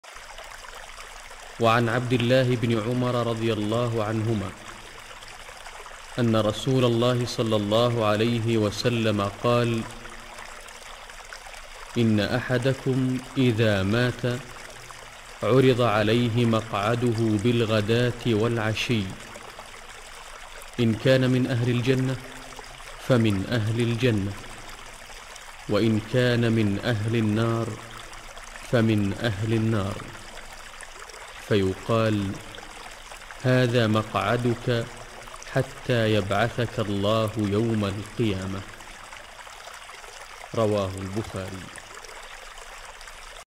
إن أحدكم إذا مات
باب عرض مقعد الميت عليه بالغداة والعشي عن نافع عن ابن عمر أن رسول الله صلى الله عليه وسلم قال إن أحدكم إذا مات عرض عليه مقعده بالغداة والعشي